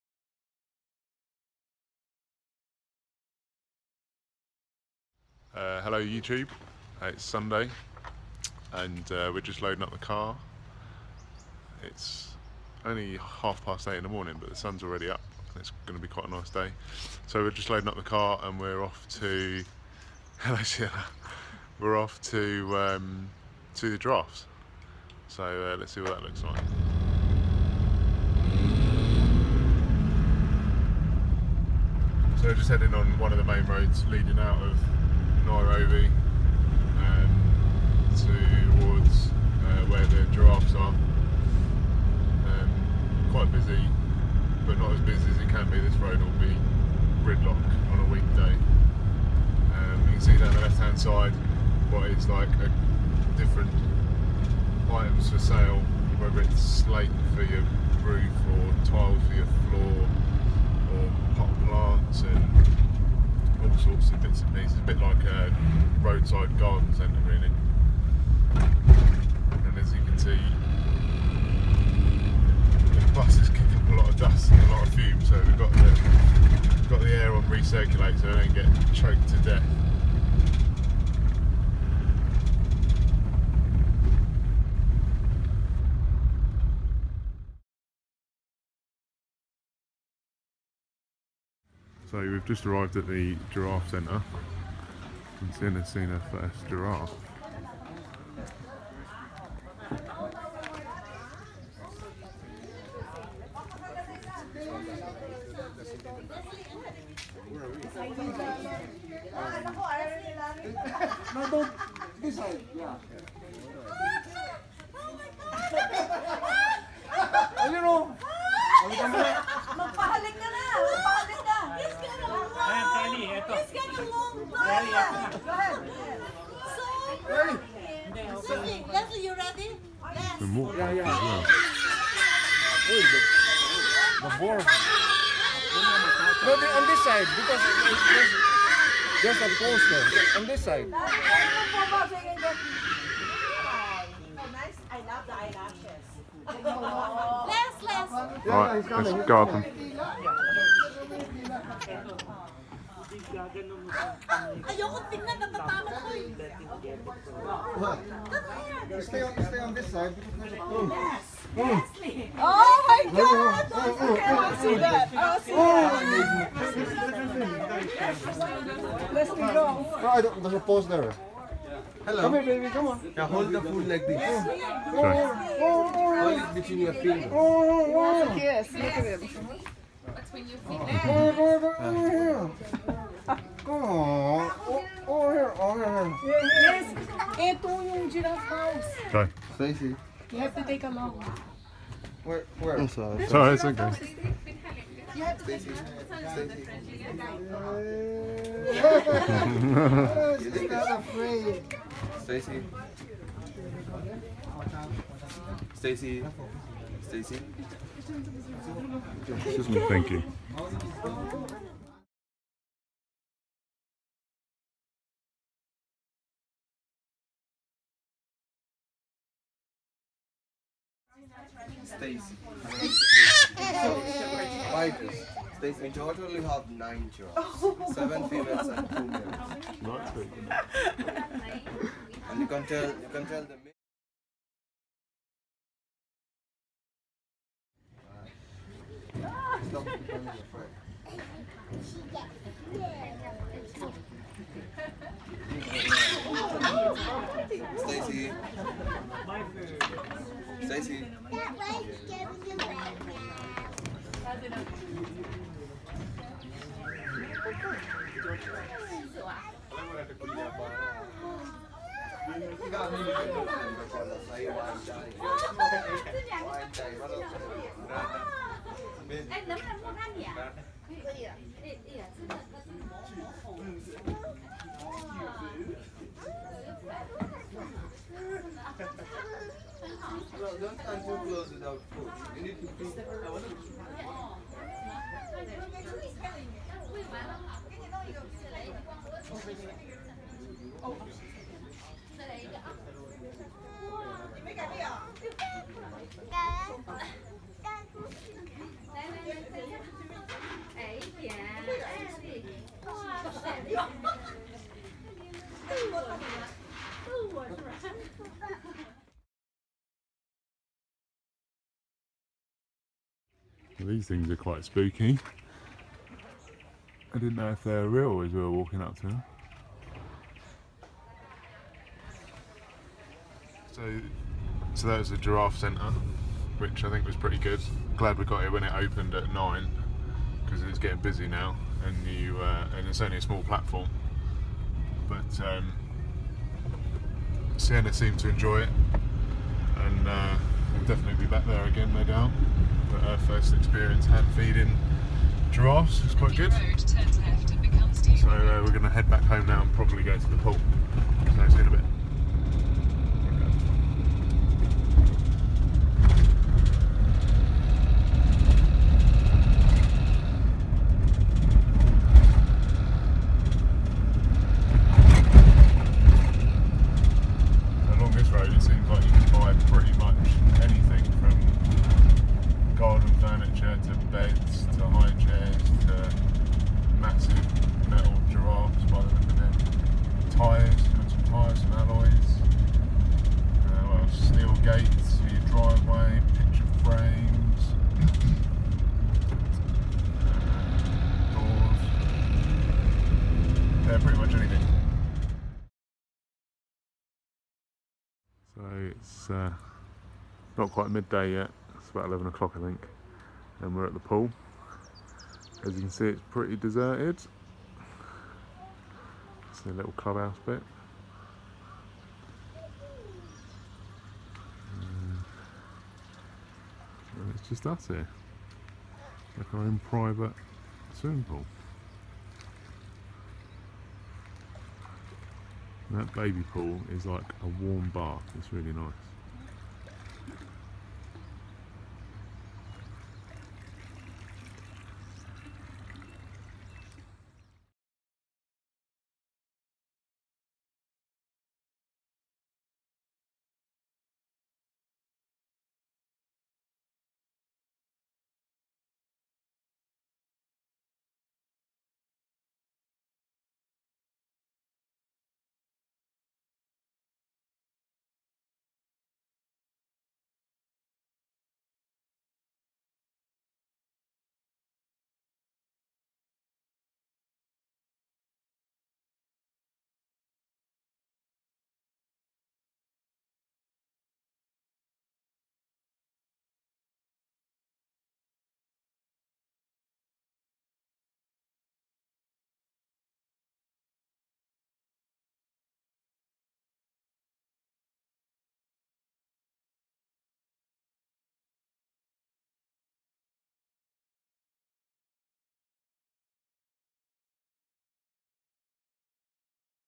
Kenyan Adventure Ep3 - Giraffes & Pool
Today we went to feed the giraffes, not bad for £3.50